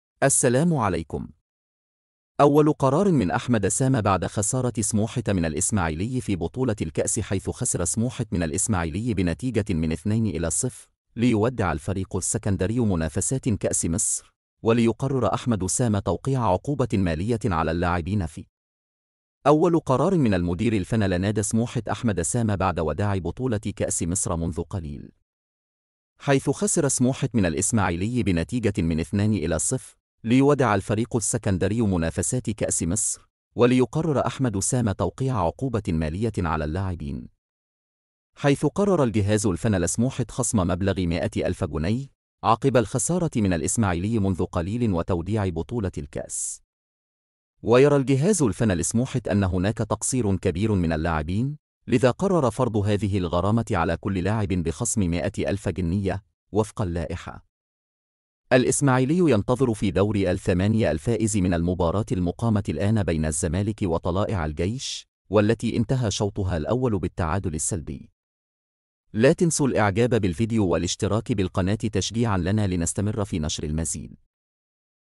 أول قرار من أحمد سامى بعد خسارة سموحة من الاسماعيلى
أول قرار من أحمد سامى بعد خسارة سموحة من الاسماعيلى فى بطولة الكأس
حيث خسر سموحة من الاسماعيلى بنتيجة 2-0، ليودع الفريق السكندرى منافسات كأس مصر، وليقرر أحمد سامى توقيع عقوبة مالية على اللاعبين
فى أول قرار من المدير الفنى لنادى سموحة أحمد سامى بعد وداع بطولة كأس مصر منذ قليل.

حيث قرر الجهاز الفنى لسموحة خصم مبلغ 100 ألف جنيه، عقب الخسارة من الاسماعيلى منذ قليل وتوديع بطولة الكأس.

ويرى الجهاز الفنى لسموحة أن هناك تقصير كبير من اللاعبين، لذا قرر فرض هذه الغرامة على كل لاعب بخصم 100 ألف جنية، وفق اللائحة.

الاسماعيلى ينتظر فى دور الـ 8 الفائز من المباراة المقامة الآن بين الزمالك وطلائع الجيش، والتي انتهى شوطها الاول بالتعادل السلبي.